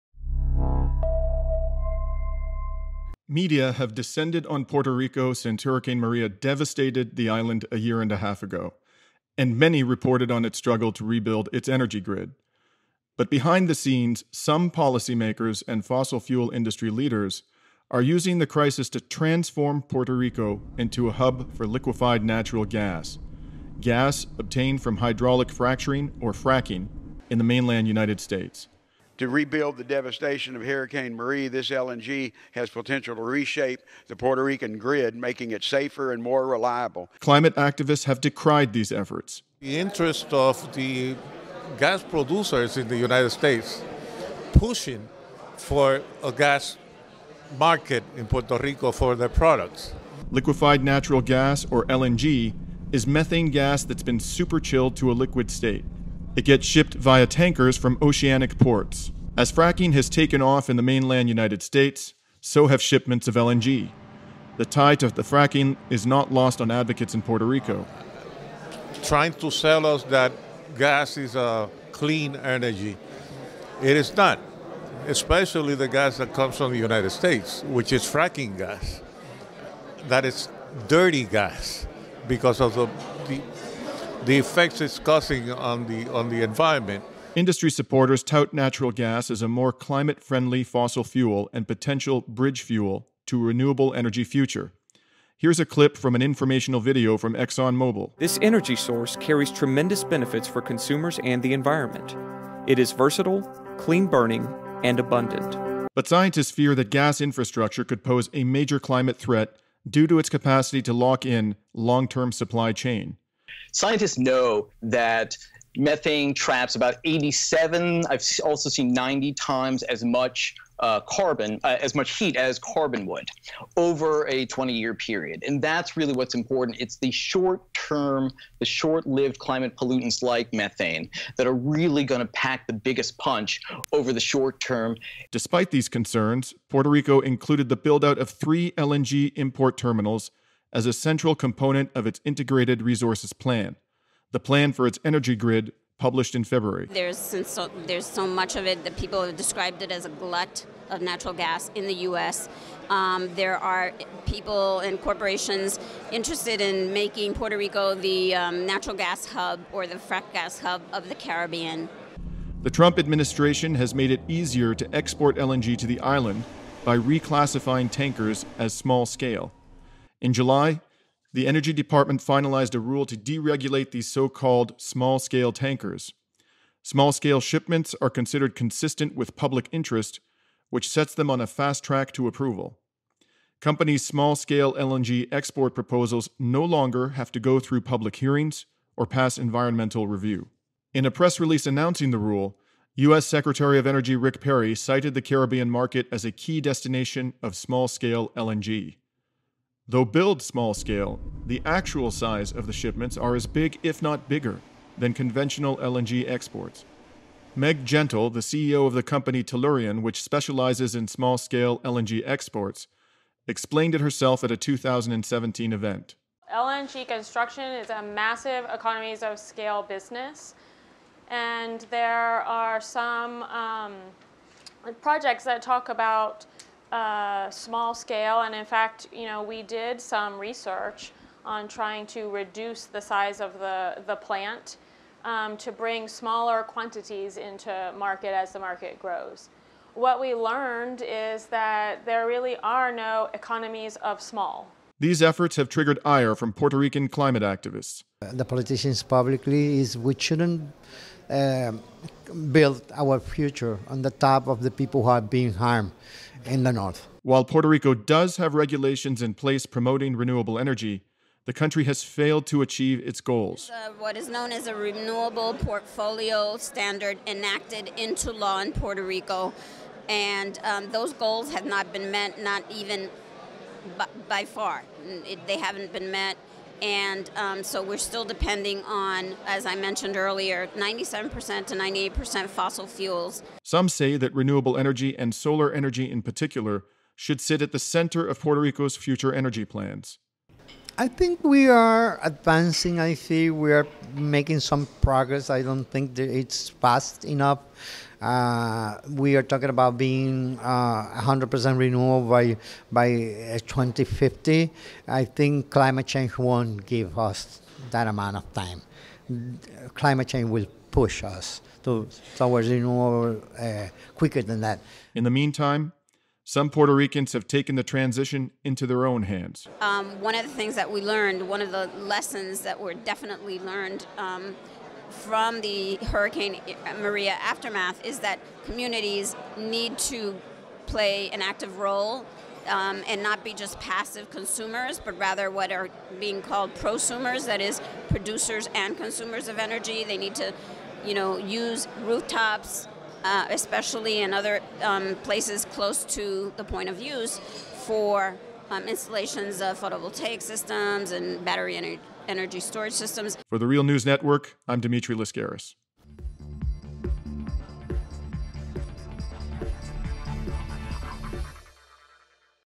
Puerto Rico Faces a Flood of Fracked Gas in Wake of Hurricane Maria
The gas industry and its allies in Congress hope to push LNG exports to the island’s electricity grid, ravaged from climate change-fueled natural disaster